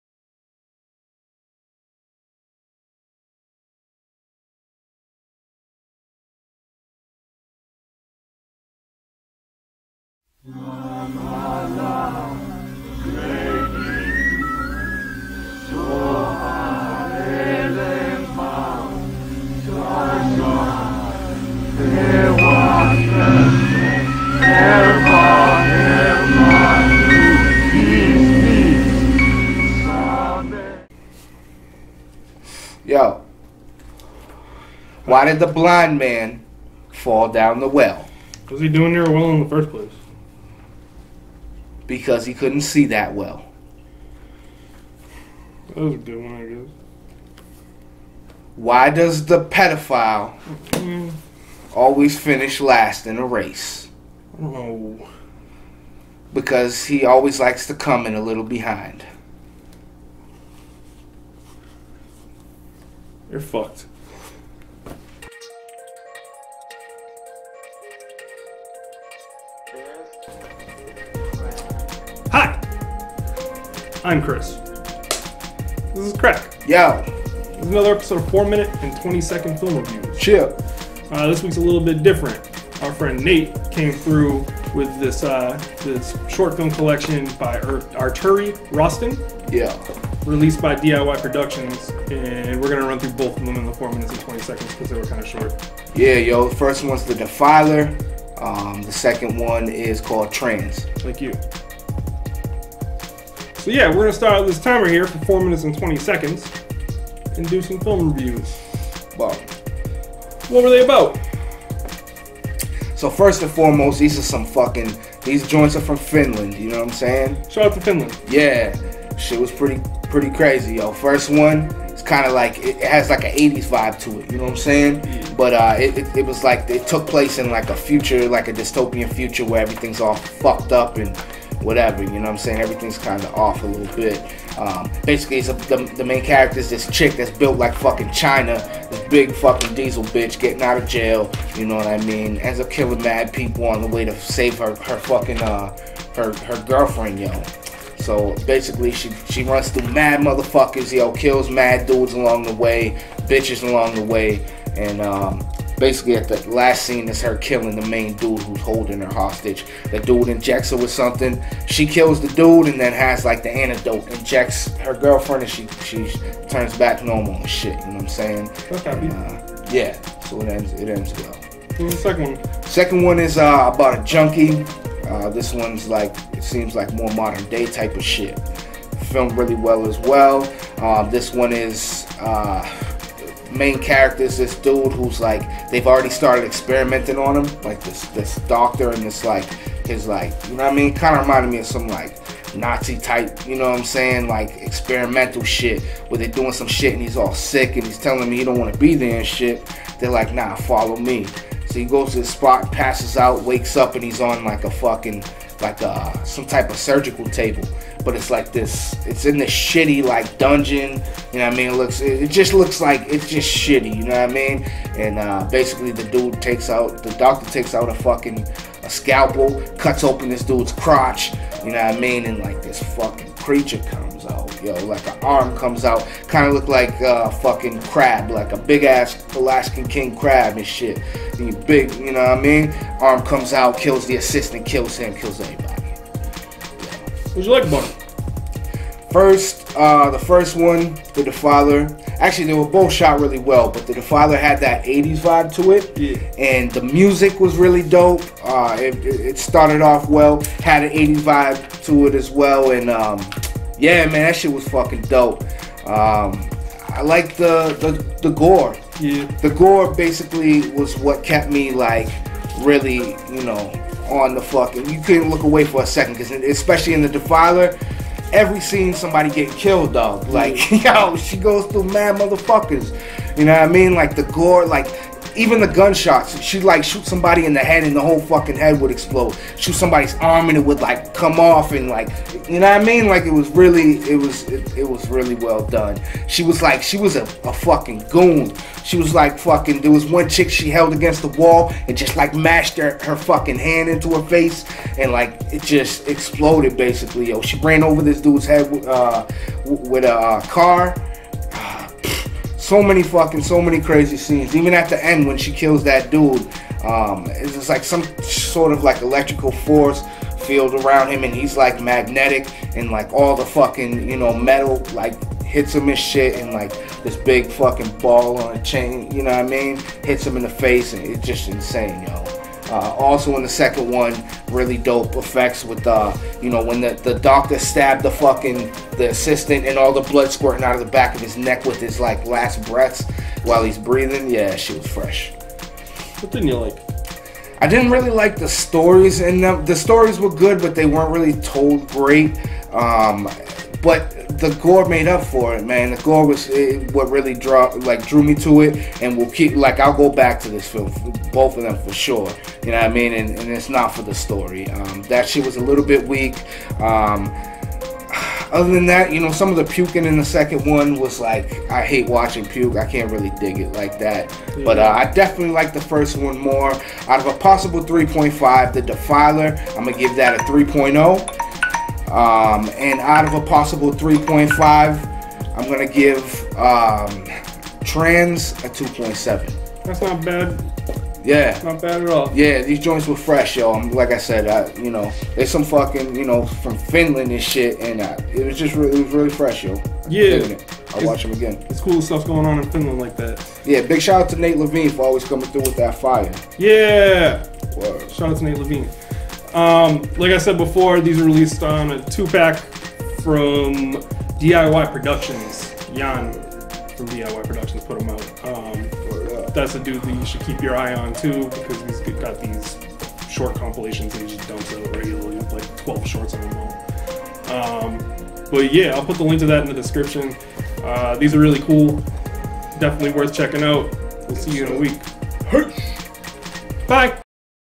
The Defiler/Trans: Four Twenty Film Review By Crack Sizzlack Episode 58
This is the fifty-eighth episode in a series of film reviews by Crack and myself. Each week we'll be watching an independent or low budget horror film and reviewing it. This week we watched THE DEFILER & TRANS. If you have any suggestions shoot us a message on the King of the Witches facebook page.

The instrumental was produced by Crack. Check out his mixtape below, you can get it for free!: